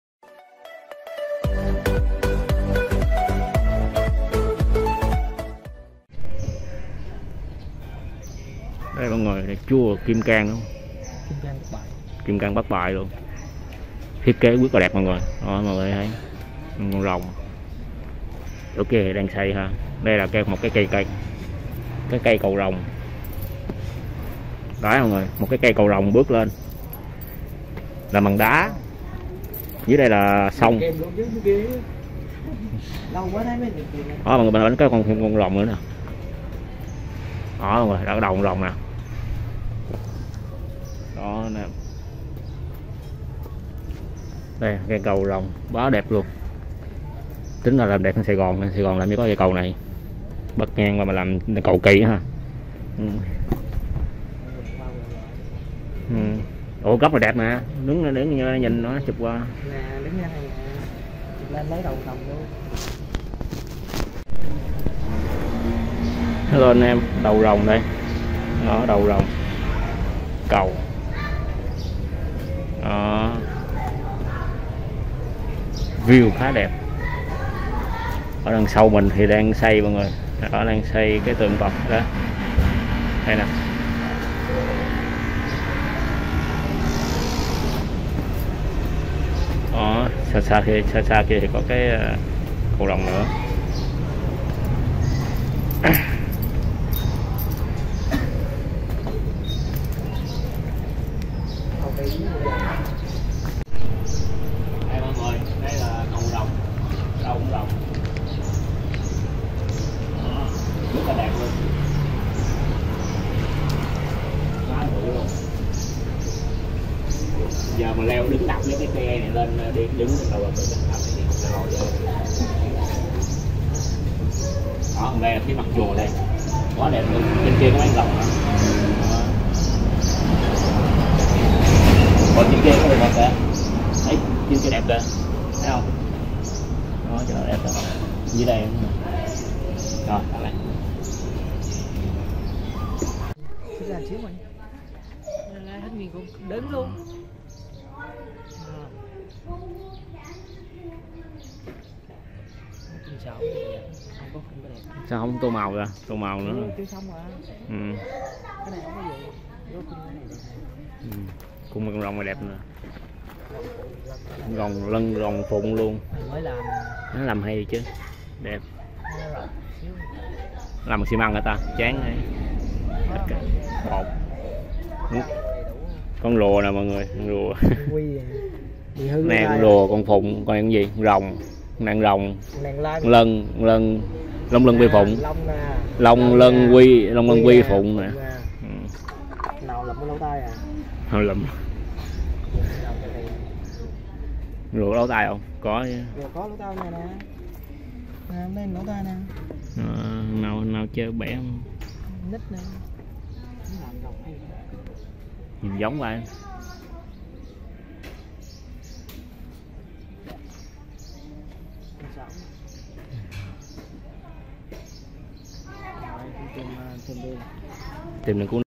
Tu Viện Kim Cang Bình Chánh Ngôi Chùa Có Cầu Rồng Rất Đẹp Cùng Hùng Khám Phá Có Gì
Ủng hộ qua VCB: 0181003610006
💸 MoMo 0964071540, Lê Cao Thanh Hùng
💸 VCB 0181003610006, Lê Cao Thanh Hùng